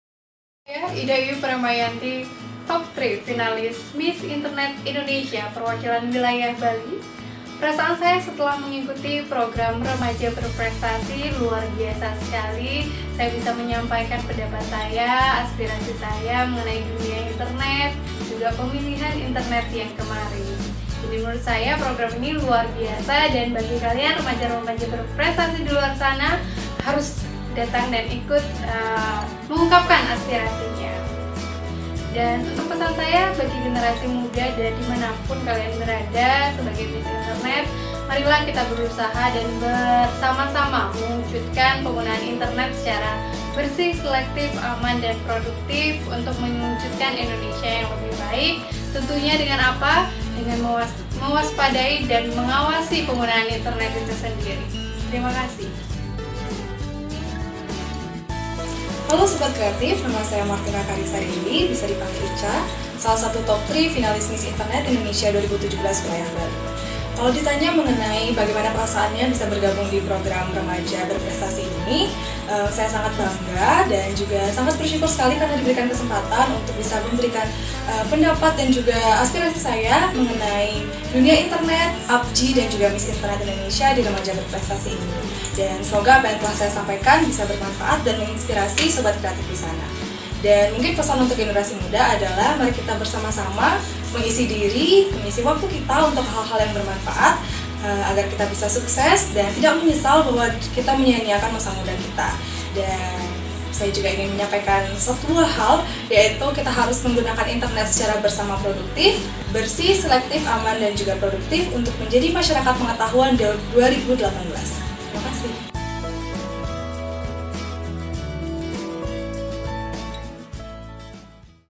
Komunitas Sadar Sehat - Vlog Ms Internet 2017
Vlog kesan Miss Internet Wilayah Bali 2017 dan juga pesan kepada generasi muda Bali